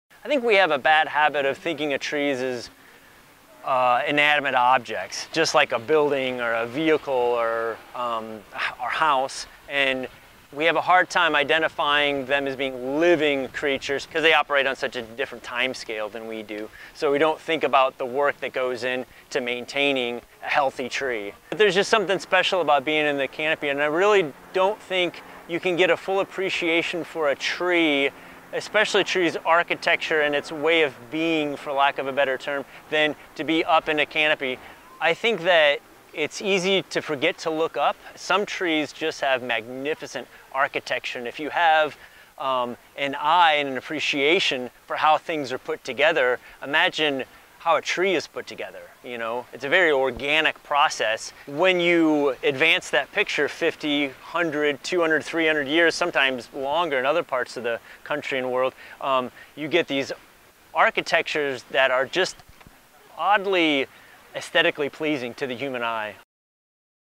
The Architecture of Trees
Missouri Botanical Garden arborist Dave Gunn describes the impressive natural architecture of trees.